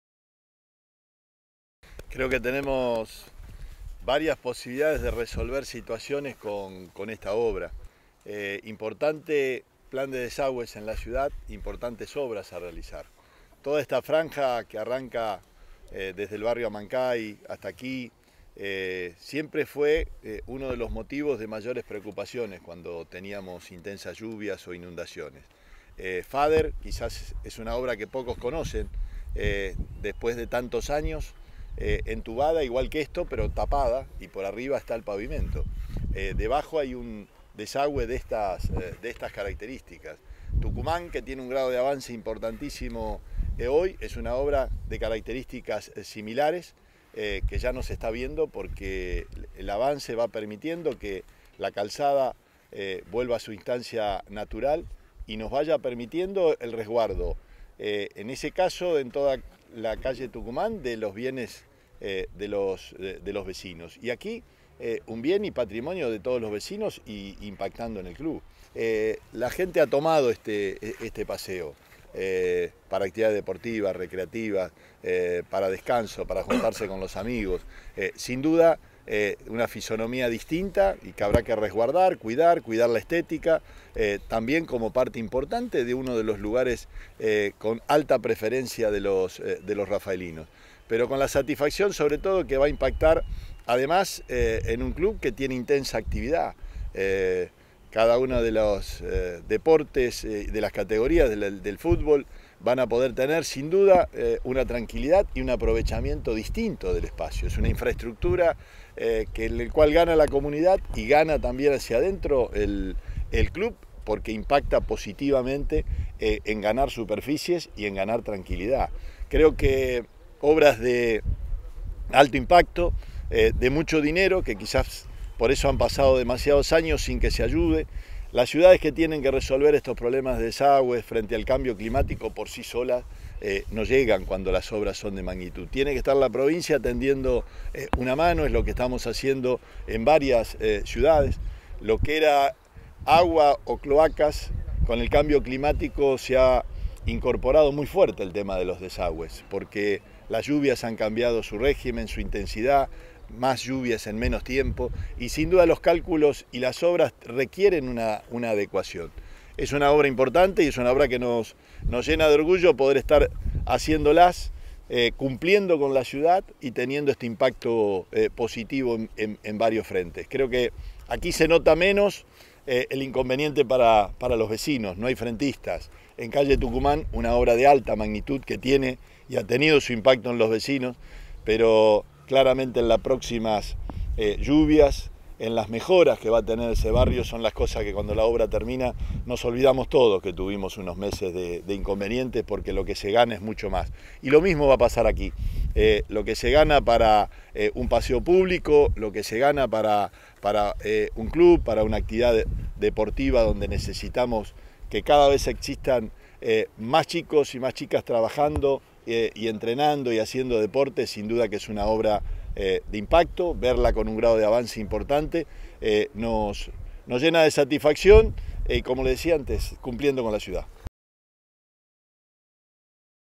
PEROTTI RECORRIÓ EN RAFAELA LA OBRA DE DESAGÜE CANAL SUR, TRAMO BEN HUR
“Son obras que cuestan mucho y que hay que hacerlas; no se ven, pero son necesarias para la calidad de vida”, dijo el gobernador. Los trabajos mejorarán las condiciones de seguridad y de aprovechamiento de los espacios recreativos.